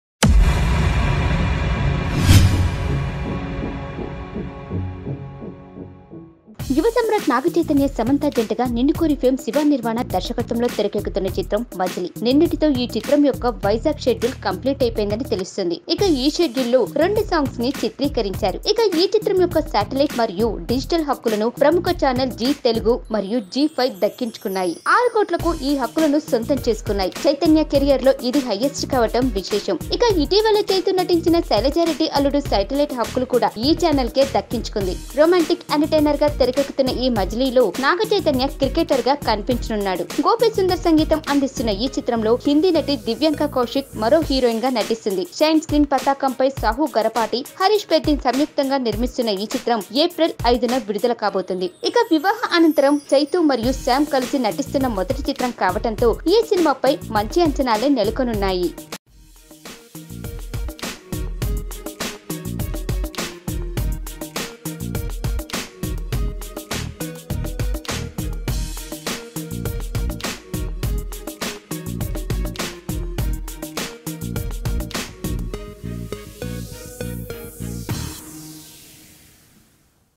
మజిలీ శాటిలైట్ రైట్స్ దక్కించుకున్న ప్రముఖ టివీ ఛానల్ |NagaChaitanya, Samantha| Latest Cinema News
Watch:మజిలీ శాటిలైట్ రైట్స్ దక్కించుకున్న ప్రముఖ టివీ ఛానల్